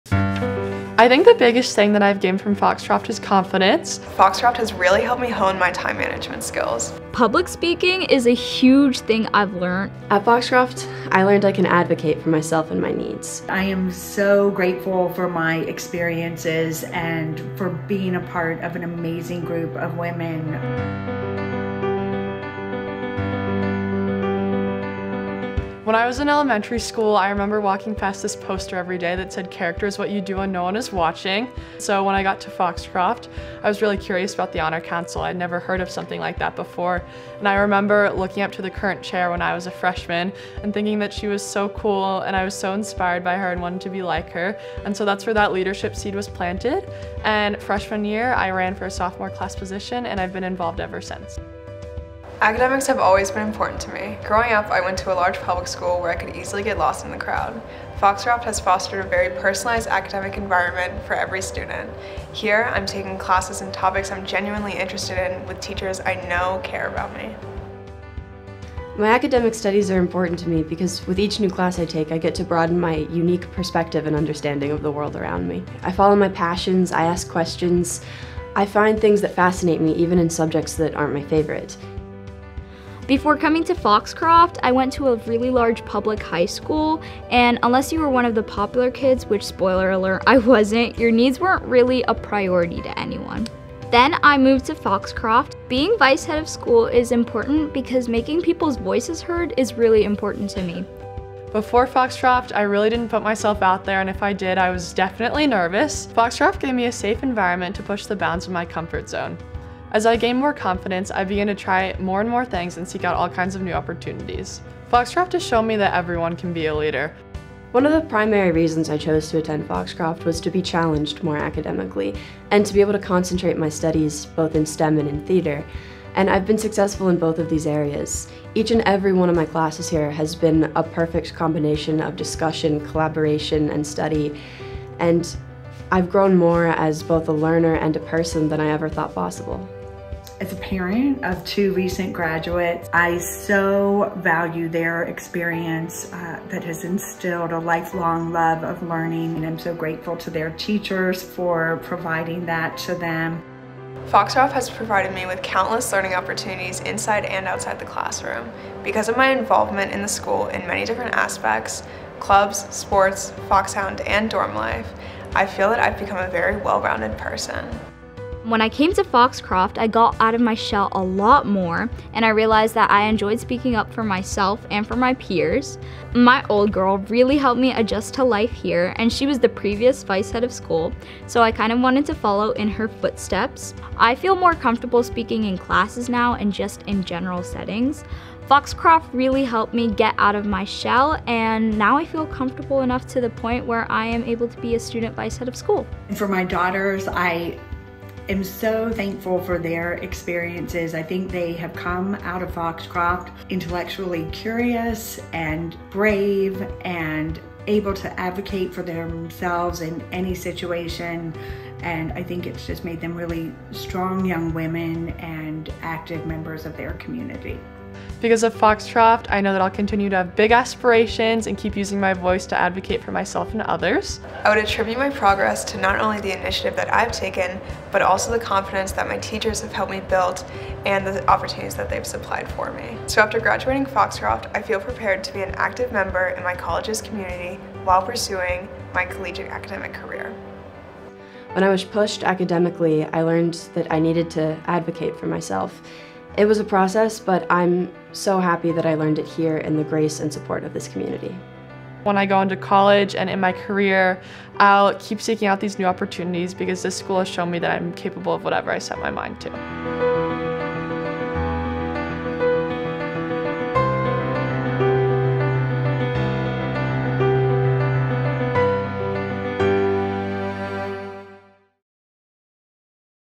What Makes the Foxcroft Experience Unique?
Hear our students share, in their own words, their individual journeys
to developing their character, intellect, and voice during their time at Foxcroft.